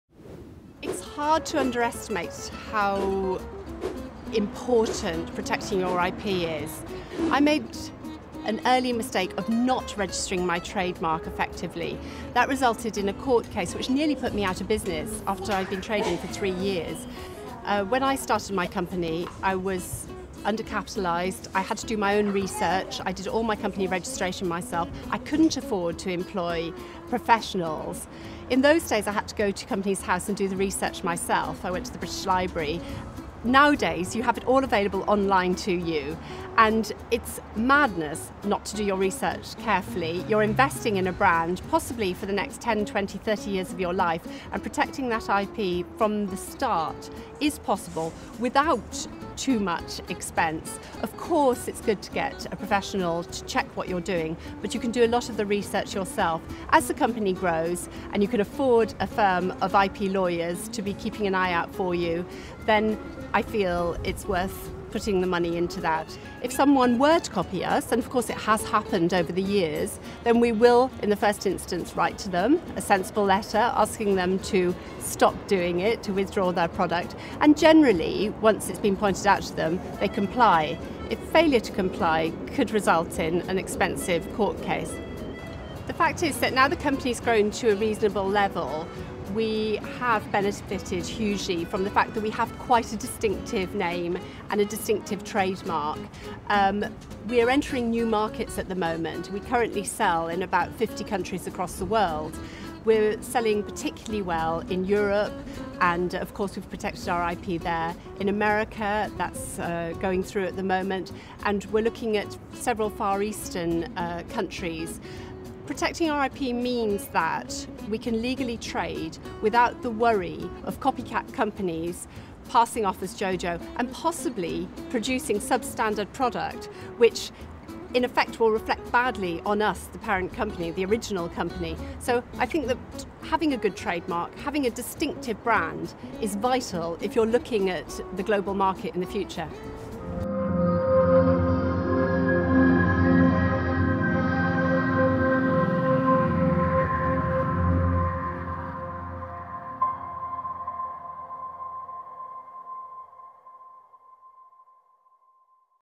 CASE STUDY: How can I build up my brand? JOJO MAMAN BEBE
Laura Tenison, owner of Jojo Maman Bebe talks about how protecting her trade mark helped grow her business.